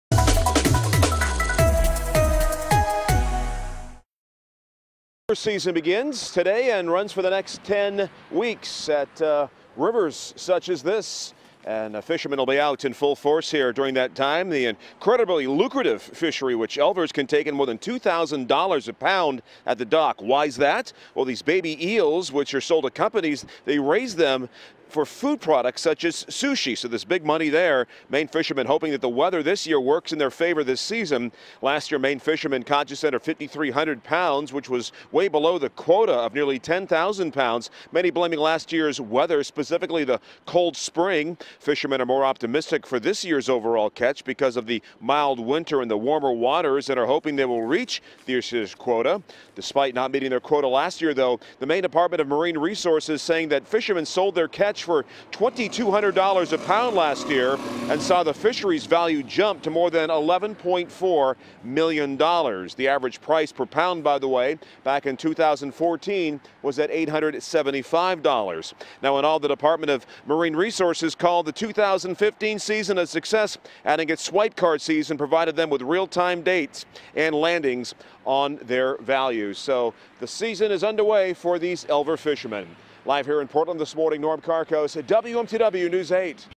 Elver fishing season underway in Maine
Maine's elver fishing season began on Tuesday. WMTW News 8's Norm Karkos reports.